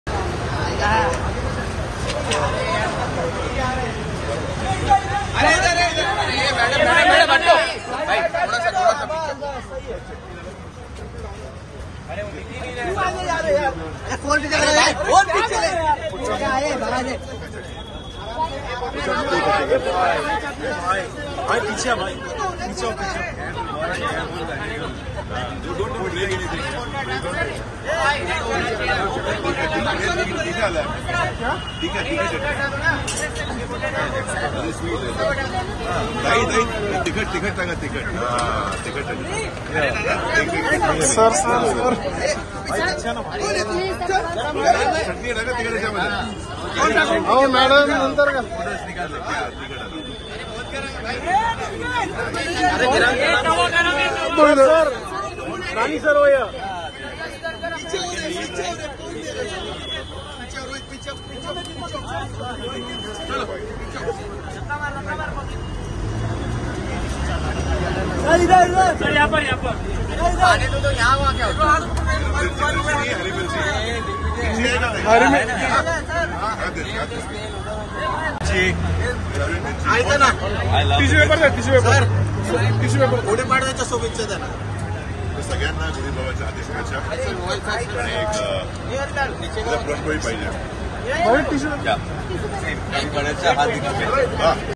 Nani And Sharad Kelkar Eating Vada Pav On The Streets Of Mumbai | Dasara Movie Promotions |News Buzz
Nani And Sharad Kelkar Eating Vada Pav On The Streets Of Mumbai | Dasara Movie Promotions